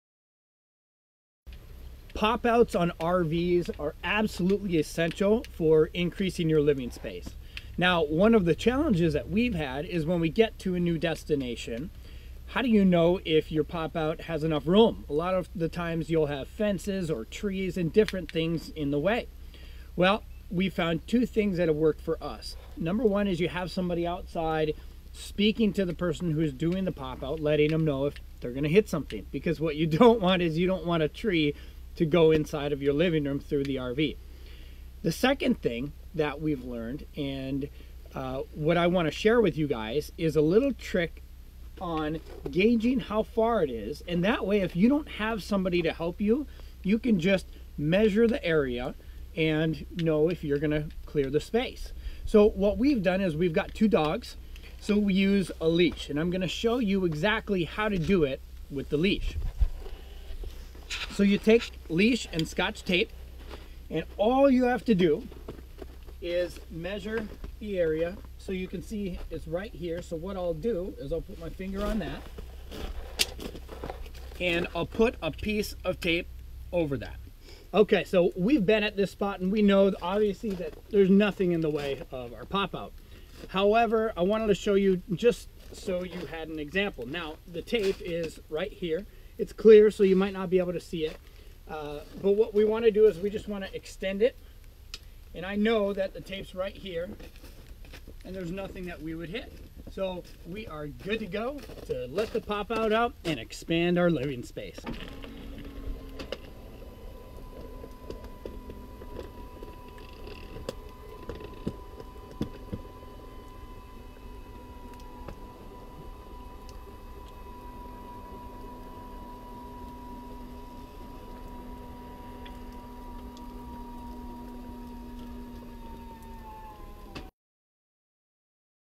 RV Popout Tip
How can you tell if your popout is going to clear the space you have at your site? Check out this little RV Hack to make your life easier.